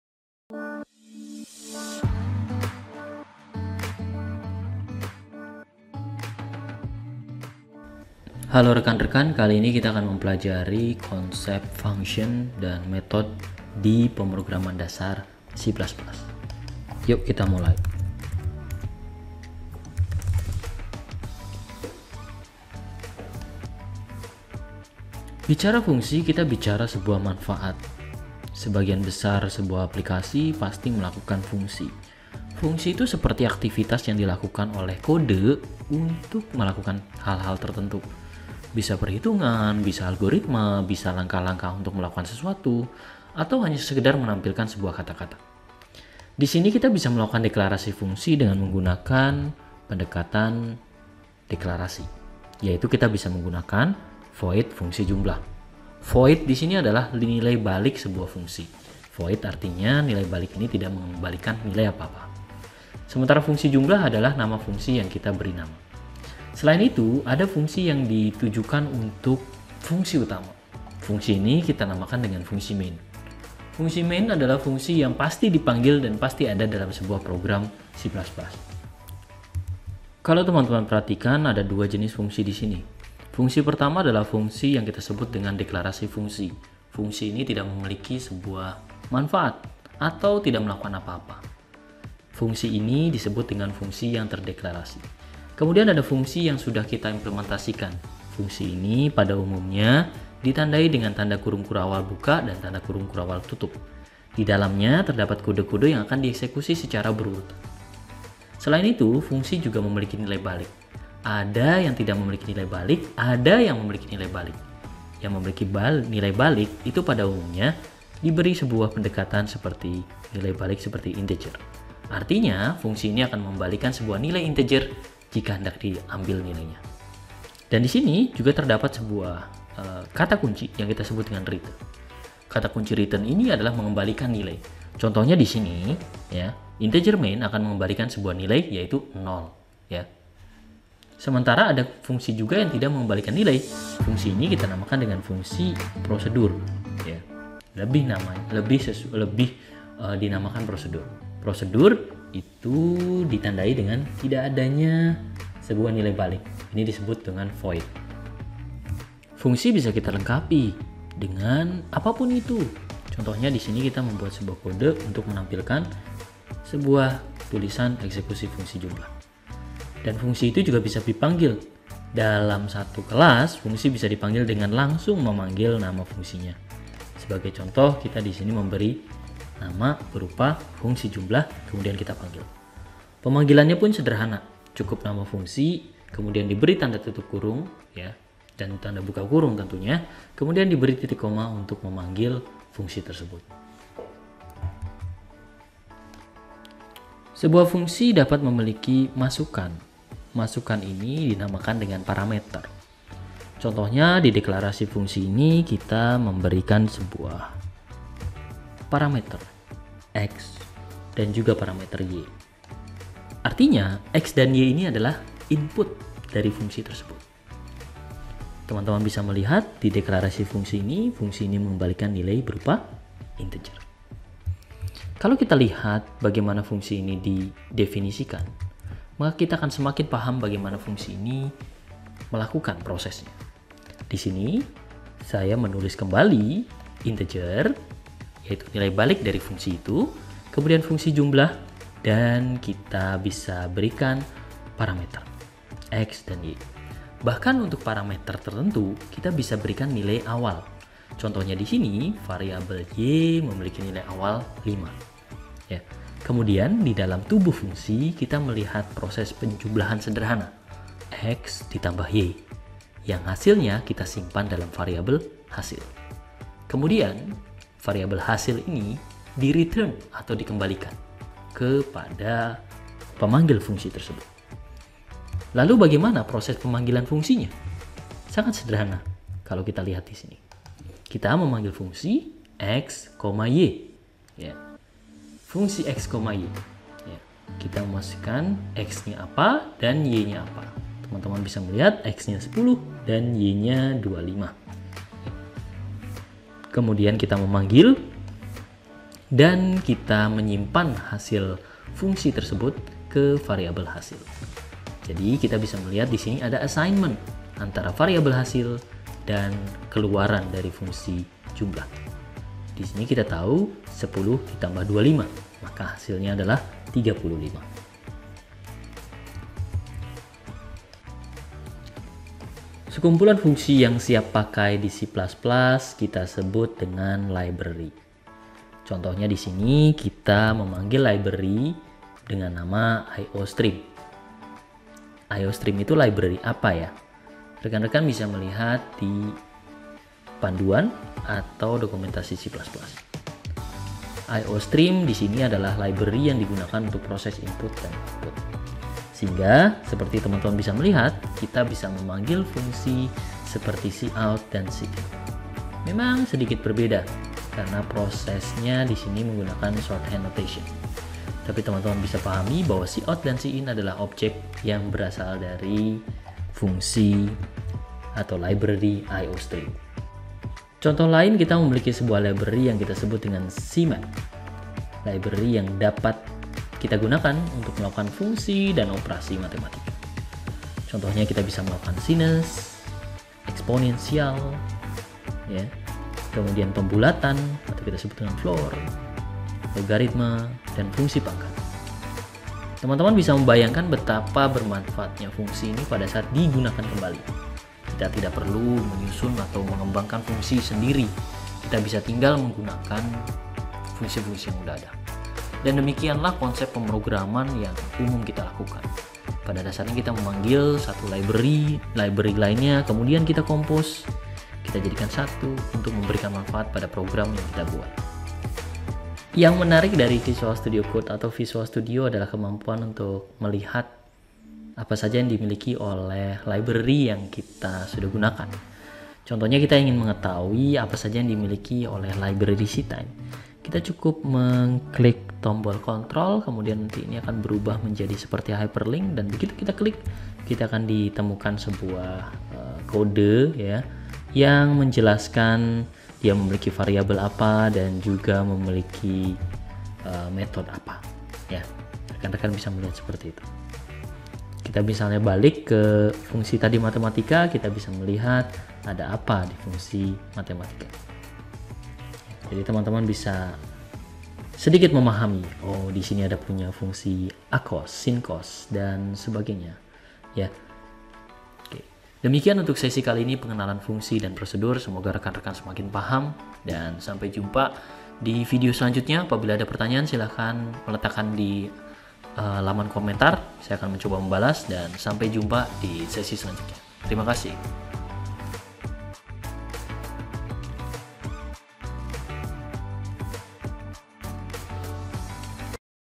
Episode 5 - Function dan Procedure
Pada video kali ini kita akan membahas konsep fungsi. Kita akan belajar bagaimana mendeklarasikan, mendefinisikan, dan menggunakan fungsi